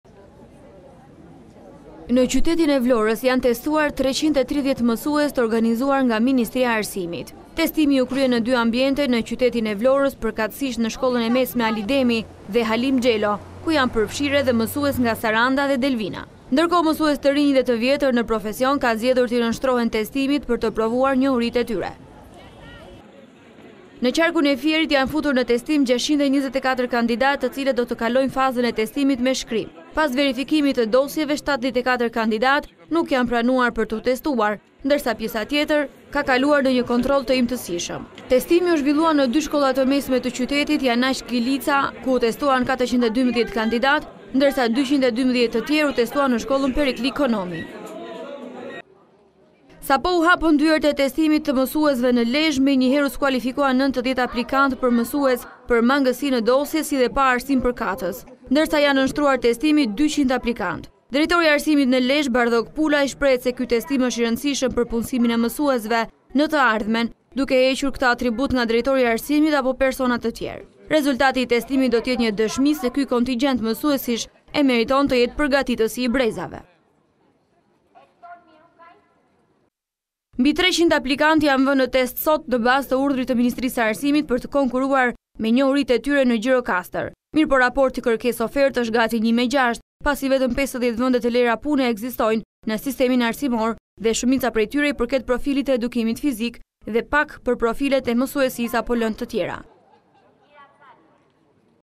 A1 Report - Testimi i mësuesve për vendet e lira, pa probleme në rrethe
Rreth 8100 mësues në të 12 qarqet e vendit i janë nënshtruar testimit kombëtar për mbi 2 mijë e 700 vende të shpallura të lira nga Ministria e Arsimit. Ku shumë prej tyre kanë aplikuar nëper rrethe.

VLORË -Në qytetin e Vlorës janë testuar 330 mësues të organizuar nga Ministria e Arsimit. Testimi u krye në dy ambinete në qytetin e Vlorës, përkatësisht shkollën e mesme “Ali Demi” dhe “Halim Xhelo”, ku janë përfshirë edhe mësues nga Saranda dhe Delvina.

Ndërkohë mësues të rinj dhe të vjetër në profesion kanë zgjedhur t’i nënshtrohen testimit për të provuar njohuritë e tyre.

FIER - Në Qarkun e Fierit janë futur për testim 624 kandidatë të cilët do të kalojnë fazën e testimit me shkrim. Pas verifikimit të dosjve 74 kandidatë nuk janë pranuar për tu testuar duke iu bërë një kontroll të imtësishëm. Testimi u zhvillua në dy shkolla të mesme të qytetit “Janaq Kilica” ku u testuan 412 kandidat dhe 212 kandidate në shkollen “Perikli Konomi”.

LEZHË - Sapo u hapën dyert e testimitë mësuesve në Lezhë, menjëherë u skualifikuan 90 aplikantë për mësues për mangësi në dosje si dhe pa arsim përkatës. Ndërsa i janë nënshtruar testimit 200 aplikante. Drejtori i arsimit në Lezhë Bardhok Pulaj shprehet se ky testim është i rëndësishëm për punësimin e mësuesve në të ardhmen duke hequr këtë atribut nga drejtori i arsimit apo personave të tjerë. Rezultati i testimit do të jetë një dëshmi se ky kontigjent mësuesisht e meriton të jetë përgatitësi i brezave.